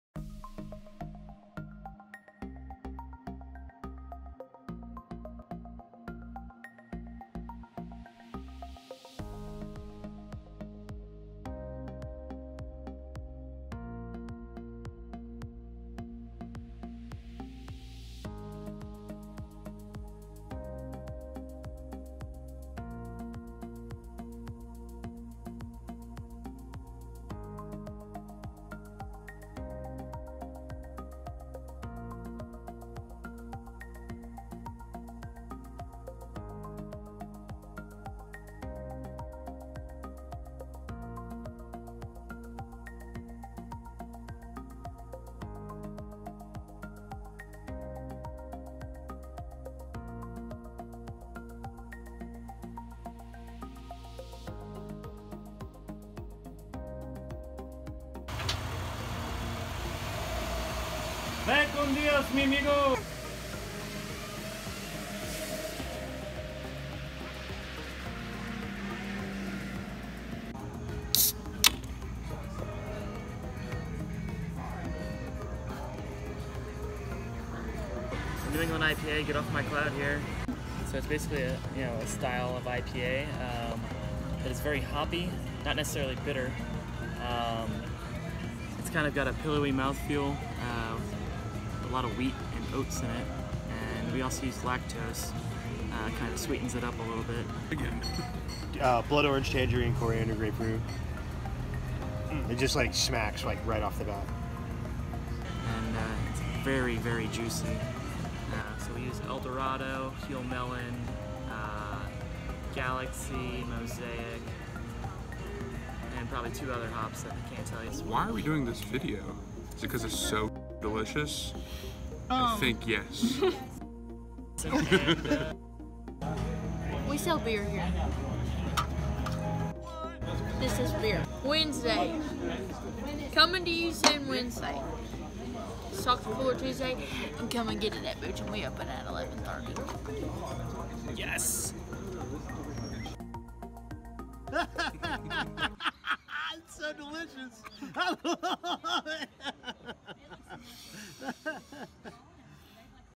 Get Off My Cloud NE IPA Packaging Day & Brewer Discussion
In the first of our new video series, we take you to the brewery for packaging day, then sit down at the taproom with our brewer to discuss Get Off My Cloud NE IPA.